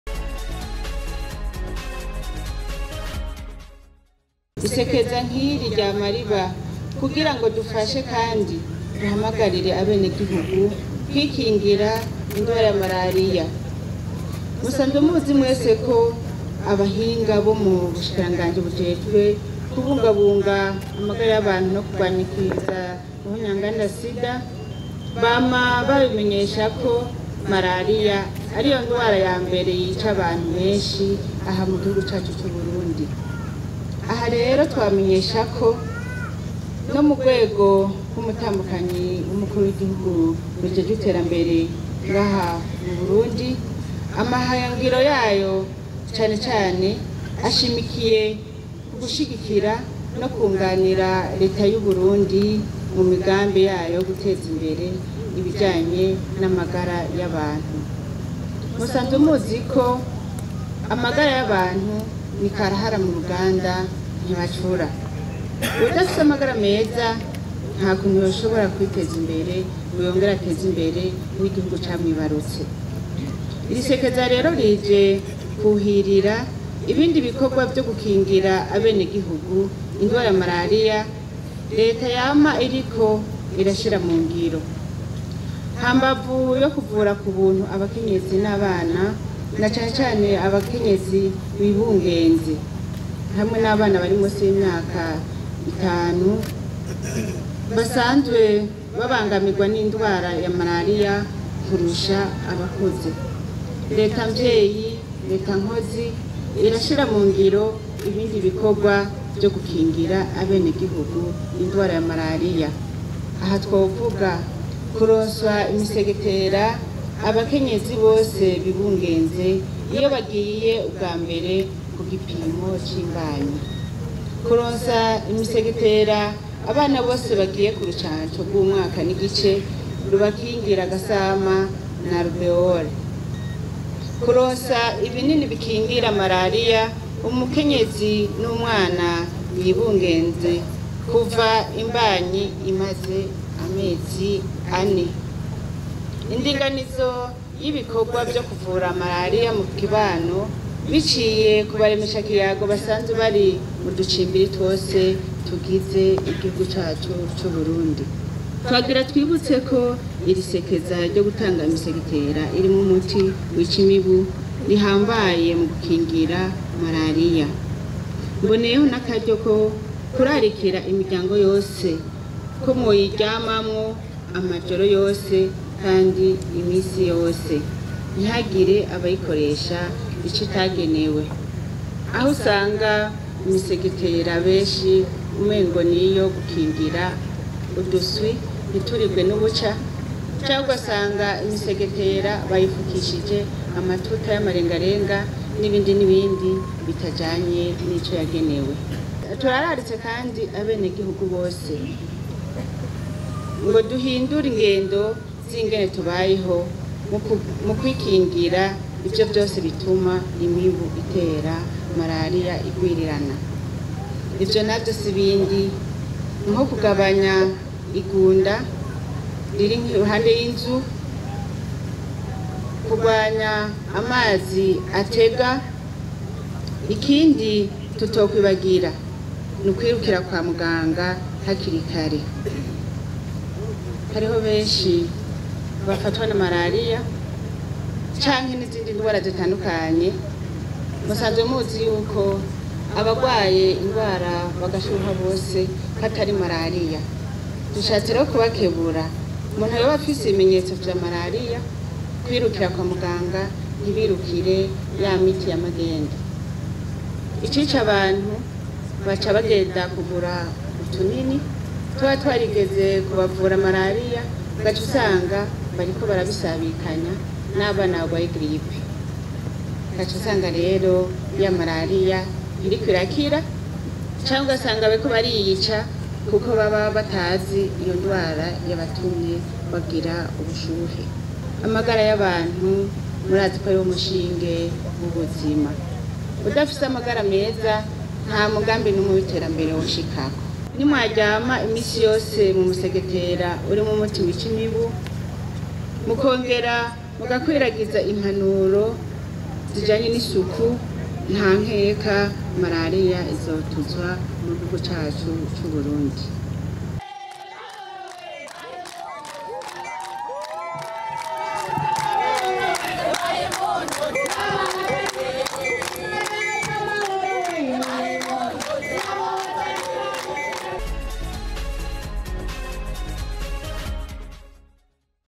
Abarundi bahamagarirwa gukoresha neza imisegetera irimwo umuti wica imibu iriko iratangwa
Abarundi bahamagarirwa gukoresha neza imisegetera irimwo umuti wica imibu iriko iratangwa mugihugu cose mw'isekeza ryo kugwanya indwara ya malariya. Ako kamo gaterwa n'umutambukanyi w'umukuru w'igihugu Aho yariko yugurura iryo sekeza i Murayi muri komine Giheta y'intara ya Gitega